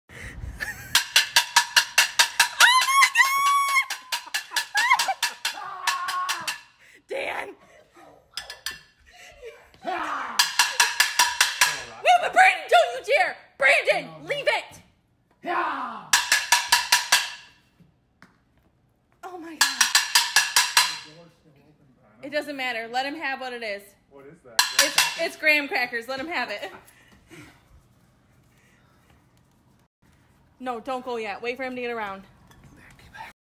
Bear Grabs the Graham Crackers || ViralHog
Occurred on May 14, 2020 / Gatlinburg, Tennessee, USA

"The bear took the box of graham crackers and ate the whole thing."